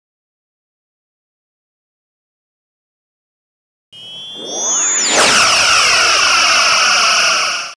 Enterprise D Firing Lower Phaser
A short render I put together using prologic9's model of the Enterprise D. All rendered in Blender Cycles and edited in Adobe Premiere CC.